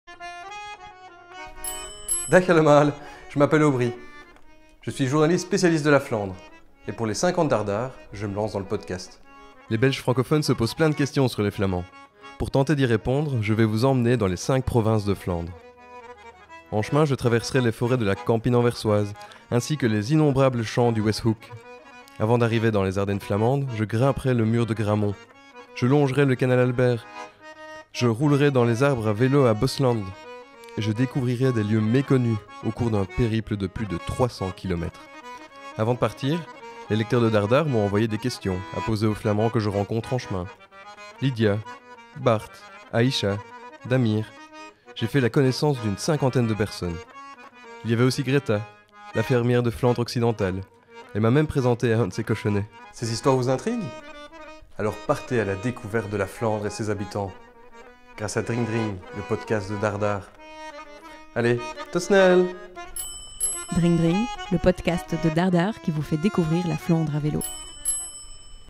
Dring Dring, le podcast qui vous fait découvrir la Flandre à vélo
Avec le podcast Dring Dring de DaarDaar, Aubry Touriel, journaliste spécialiste du nord du pays, vous emmène en voyage à vélo à travers la Flandre à la rencontre de ses habitants.

Réalisation: Adrienne Popovic & Aubry Touriel
Photos: Jef Van den Bossche
Illustration: Camille Toussaint